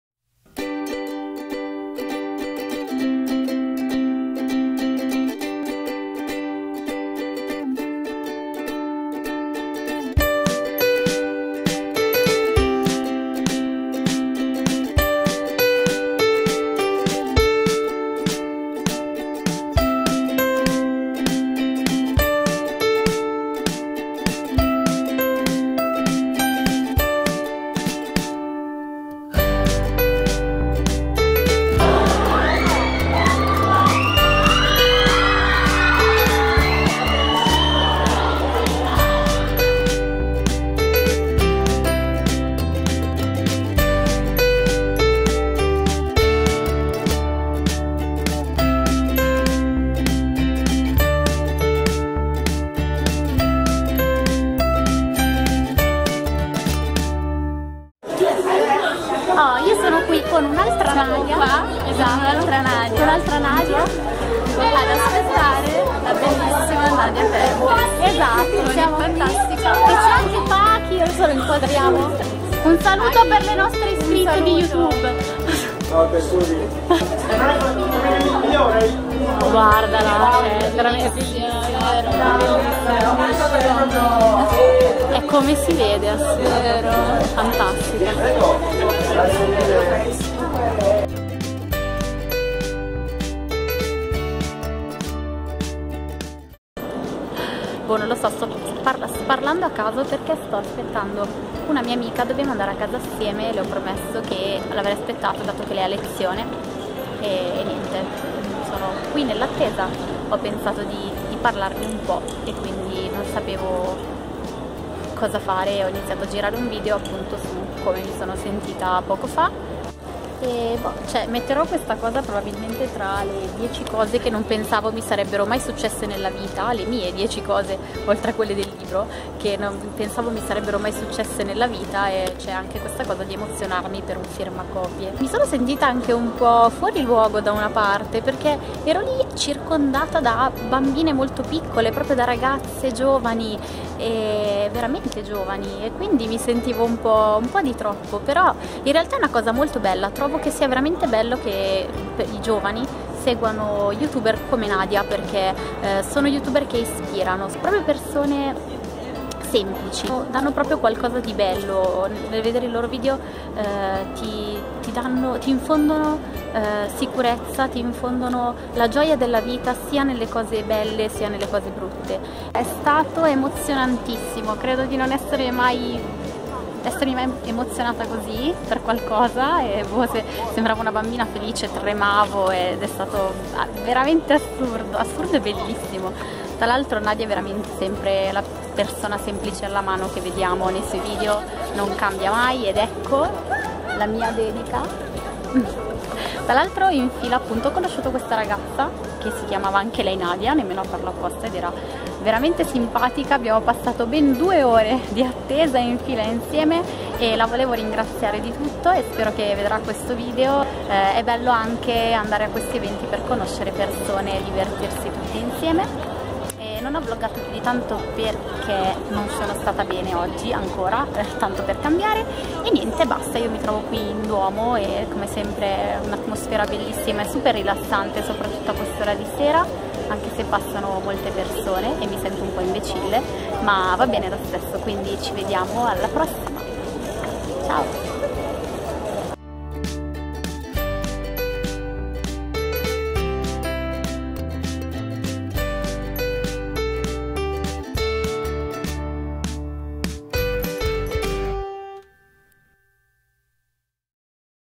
Incontro NadiaTempest a Milano
Ho incontrato una delle mie vlogger preferite a Milano in occasione del firmacopie del suo primo libro. Mi sono emozionata molto, quindi il video fa un po' schifo...